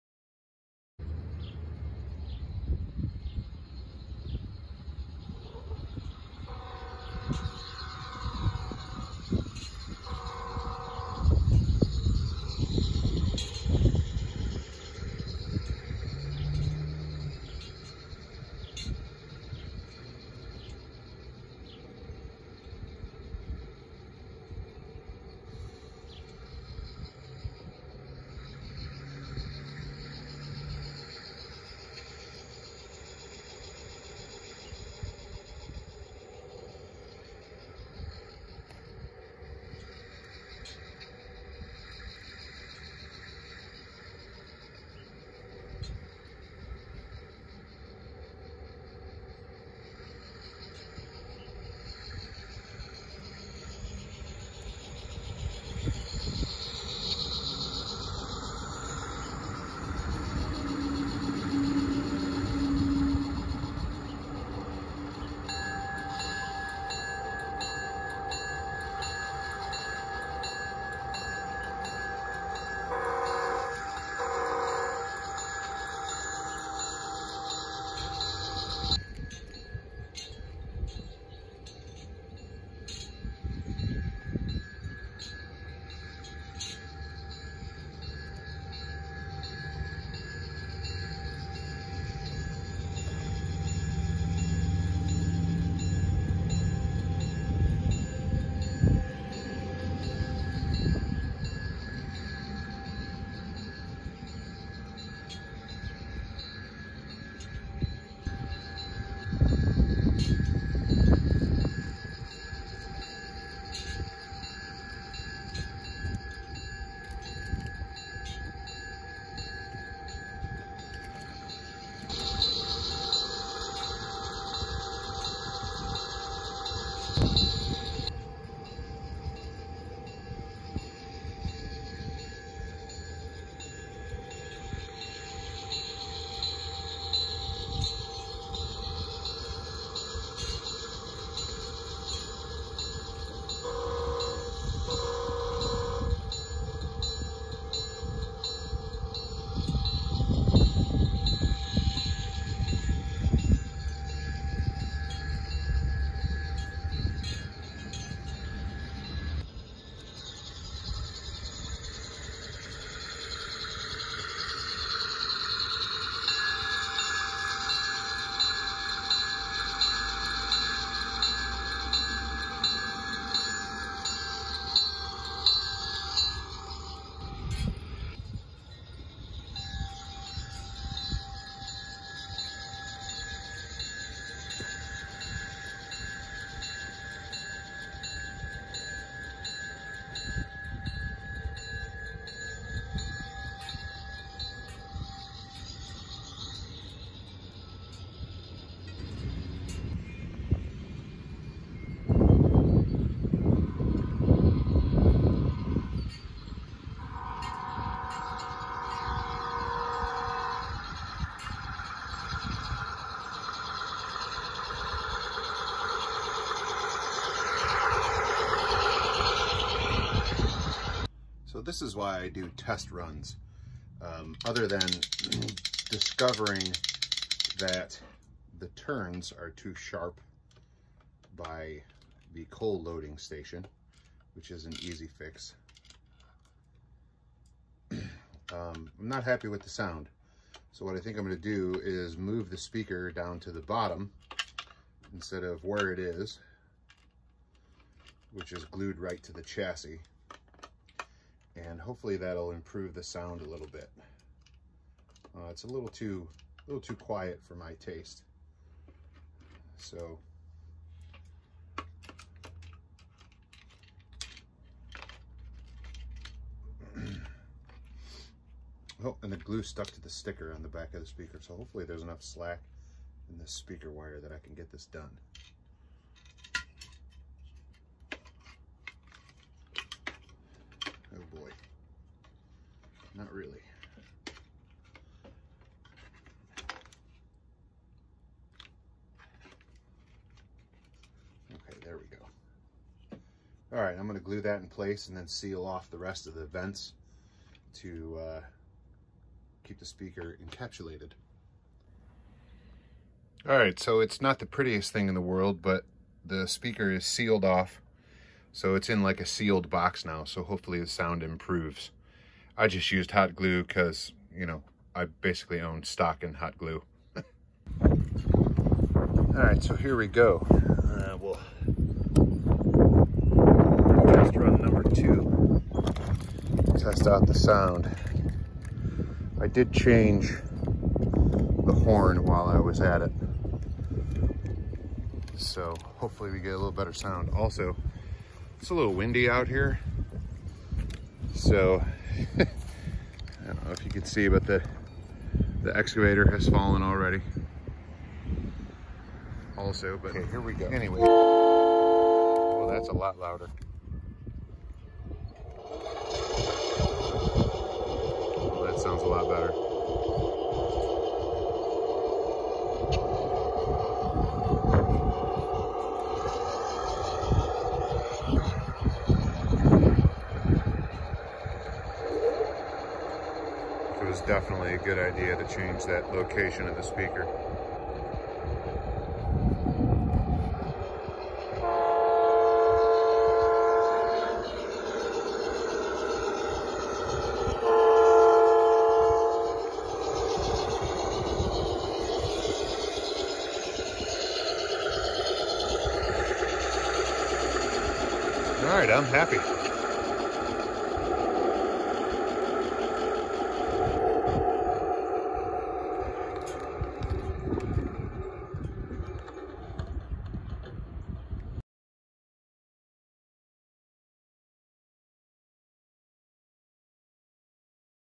G Scale USA Trains Santa Fe NW-2, Railpro and MTH Remote Couplers Test Run 3/30/21
Happy to my gorgeous wife!